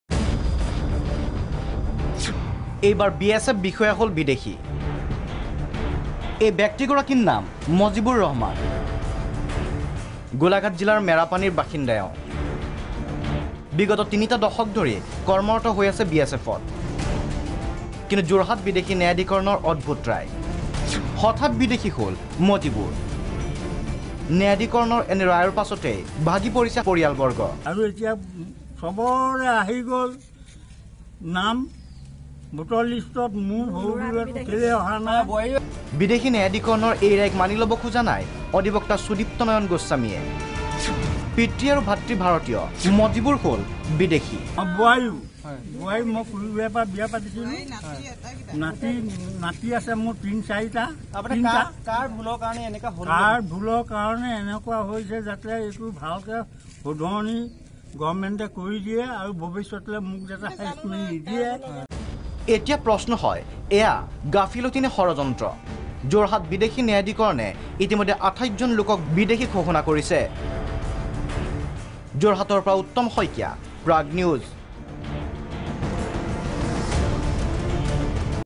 BSF jawan now declared "Foreigner" by Foriegners Tribunal
A BSF jawan who served his nation by protecting his countrymen and guarding the borders from enemy faction has been declared by Foreigners Tribunal.